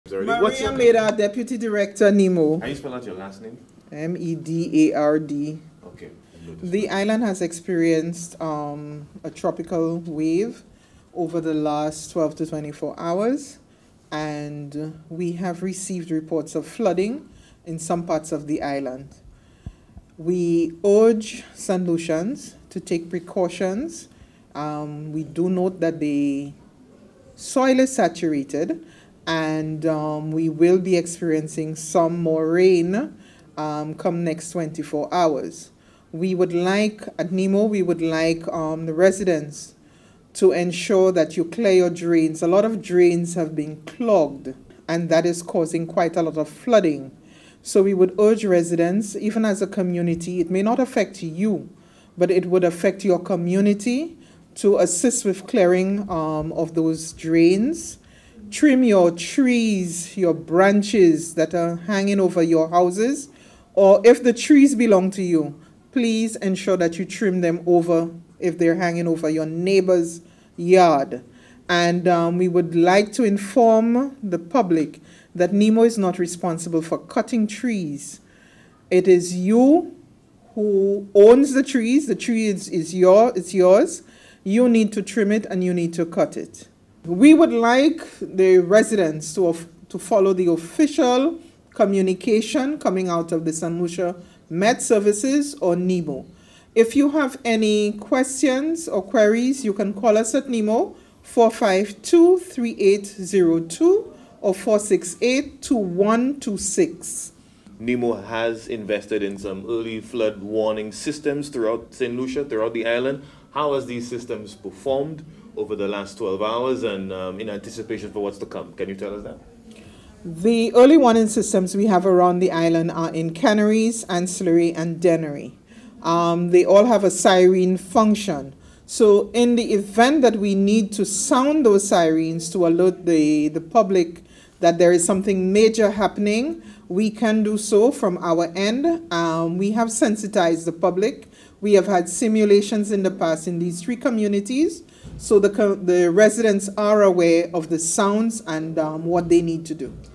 Weather update by Deputy Director of NEMO, Maria Medard
The National Emergency Management Organization (NEMO) urges the public to ensure drains and waterways near their homes remain clear of debris to reduce flooding risks during weather systems including storms and hurricanes.